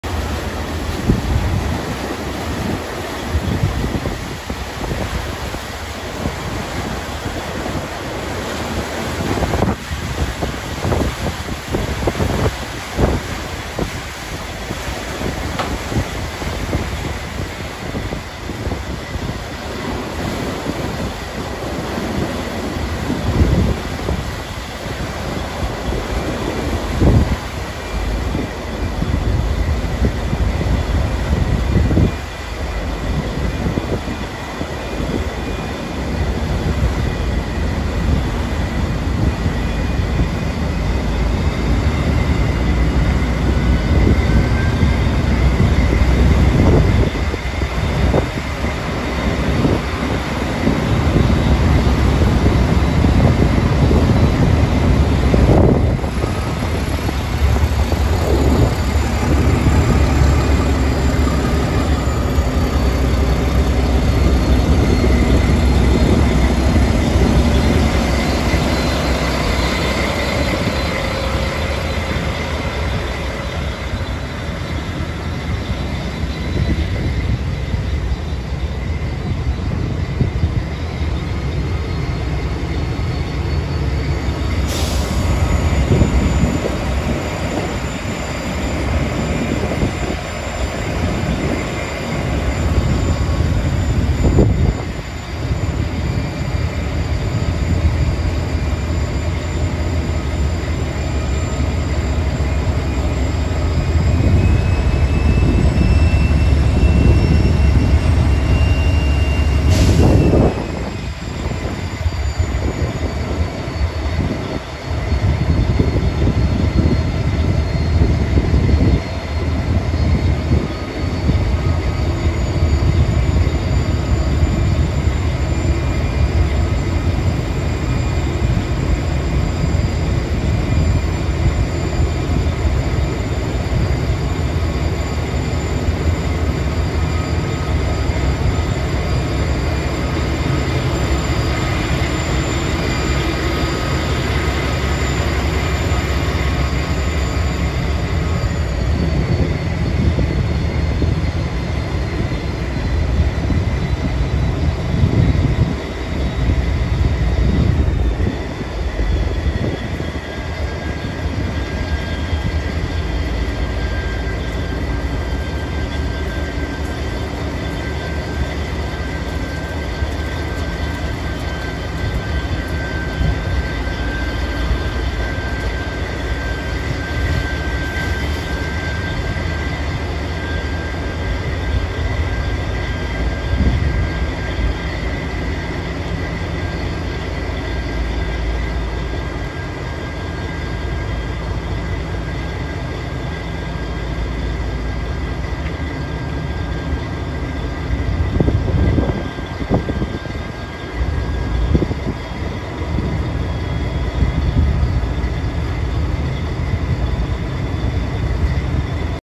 Binnenschiff Charly: SCHIFFSHEBEWERK LÜNEBURG | SCHARNEBECK
Direkt auf einem Binnenschiff aufgenommen beim Hebevorgang.